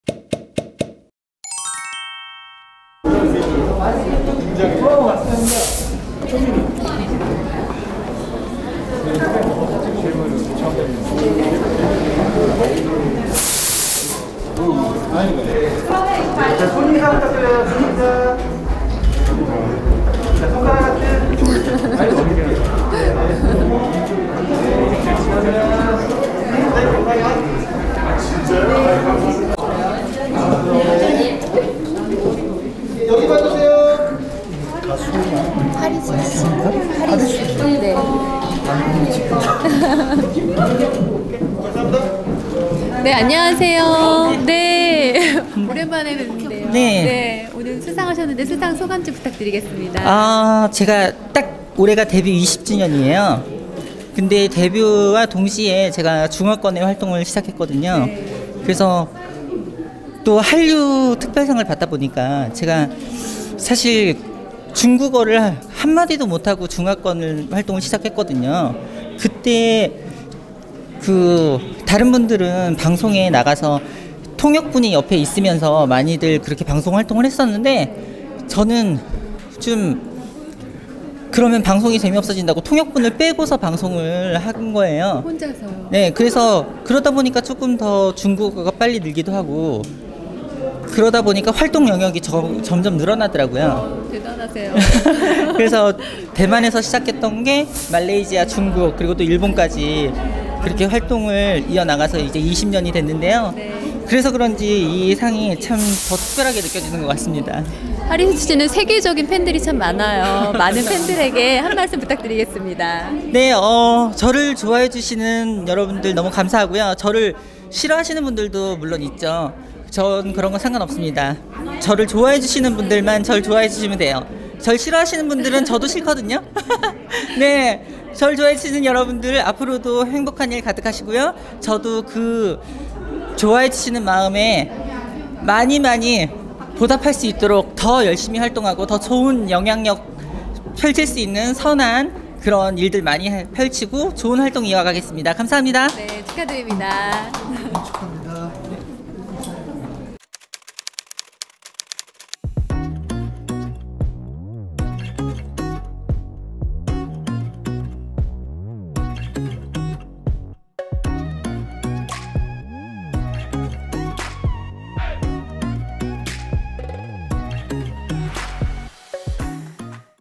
하리수, ‘여전한 미모~’ (@2021월드스타연예대상 포토월) [O영상]
하리수가 21일 오후 경기도 고양시 일산동구 장항동 빛마루 방송지원센터에서 진행된 ‘2021 월드스타연예대상’ 시상식에 참석해 포토타임을 가지고 있다.

한편, 하리수는 ‘2021 월드스타연예대상’에서 한류부문 특별상을 수상했다.